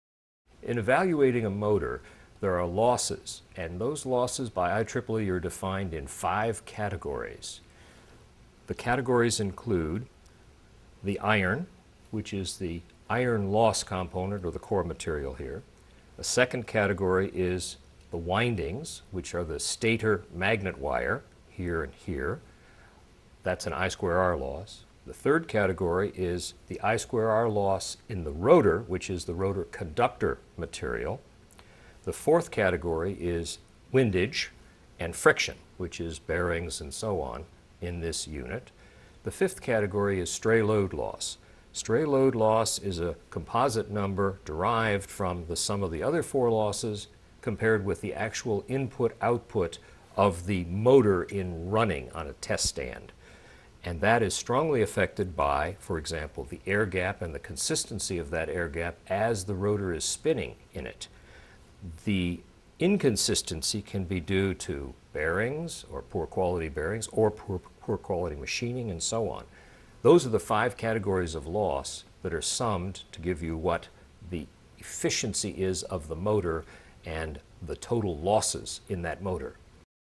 Motor Loss Categories
Dr. Edwin Brush of BBF and Associates explains the loss categories in an electric motor using a  motor cutaway.